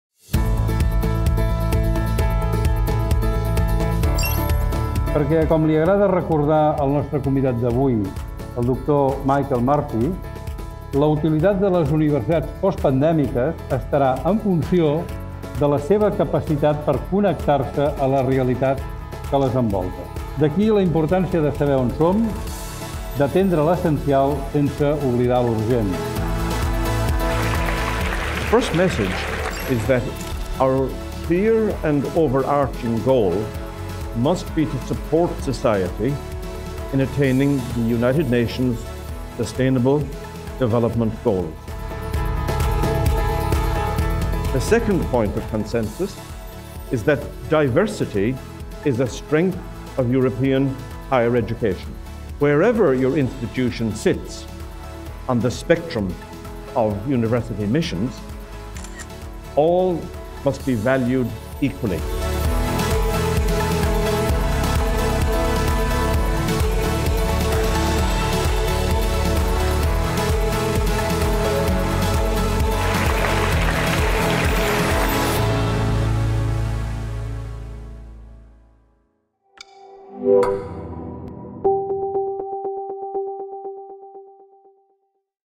Making of Lliçó inaugural amb Michael Murphy 2022 I UOC
Michael Murphy es l’encarregat d’obrir el 27è. curs de la UOC amb la lliçó inaugural “Universitats sense murs. L'adaptació al canvi a Europa”.

Metge de professió, Murphy és president de l'Associació d'Universitats Europees (EUA, per la sigla en anglès) des del 2019 i president emèrit de la University College Cork des del 2017 (en fou president durant els 10 anys anteriors, del 2007 al 2017).

Amb una àmplia trajectòria desenvolupant càrrecs de lideratge tant a Irlanda com a nivell internacional, ha estat vicerector de la Universitat Nacional d’Irlanda, membre del Consell de l’Associació Europea d’Universitats, president del Consell de Recerca Sanitària (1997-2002) i membre del Consell de l’Organisme de Serveis de Salut (2003-209).

Participen a l’acte el rector de la UOC, Josep A. Planell; el president del patronat de la UOC, Pere Vallès, i la consellera de Recerca i Universitats de la Generalitat de Catalunya, Gemma Geis.

ENG

✔ Our programmes ¬- bachelor's degrees, master's degrees, postgraduate programmes, seminars and open courses